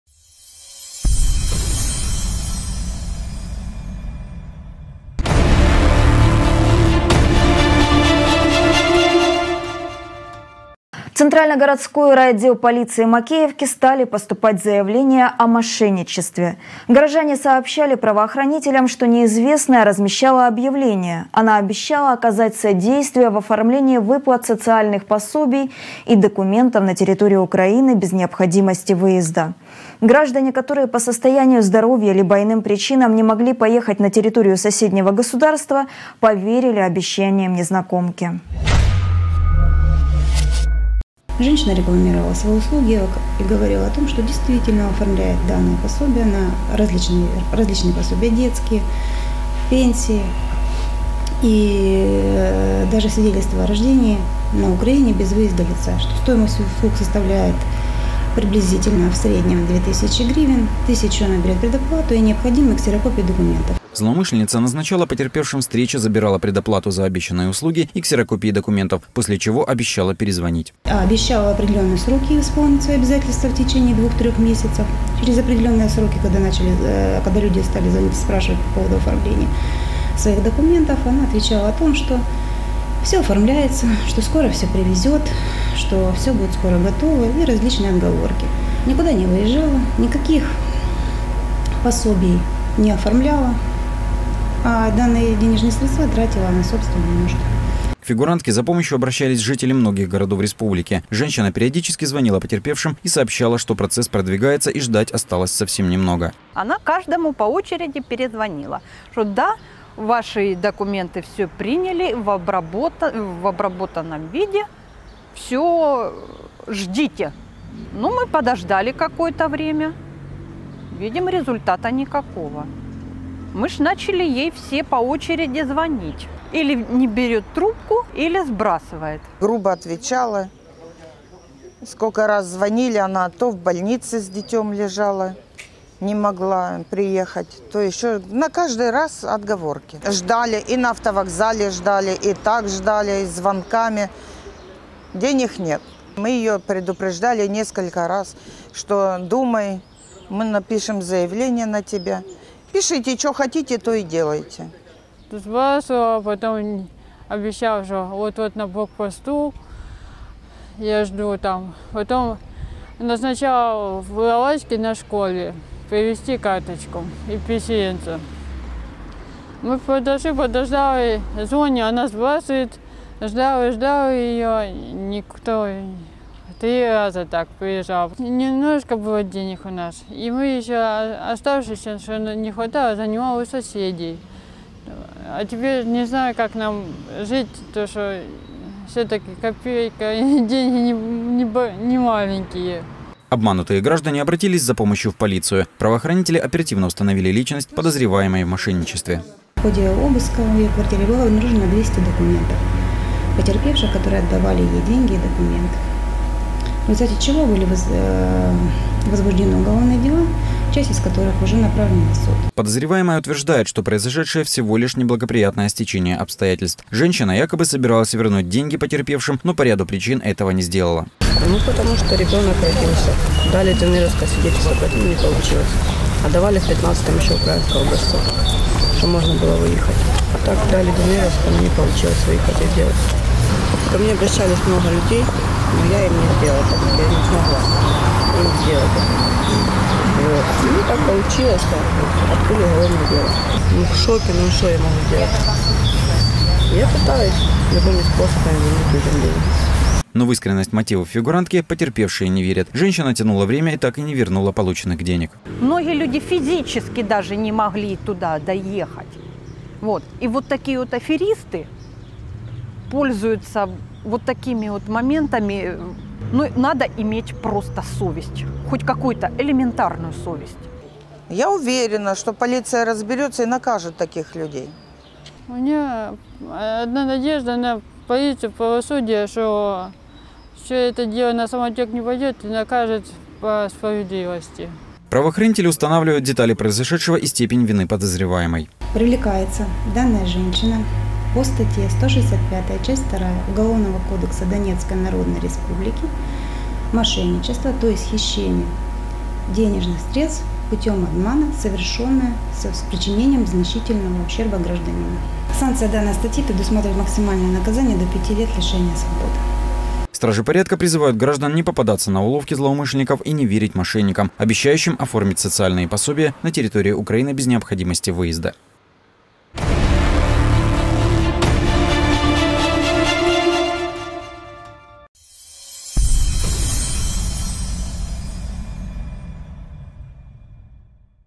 В Макеевке задержана мошенница
В Центрально-Городской райотдел полиции Макеевки стали поступать заявления о мошенничестве. Горожане сообщали правоохранителям, что неизвестная размещала объявления. Она обещала оказать содействие в оформлении выплат социальных пособий и документов на территории Украины без необходимости выезда.   Граждане, которые по состоянию здоровья либо иным причинам не могли поехать на территорию соседнего государства, поверили обещаниям незнакомки.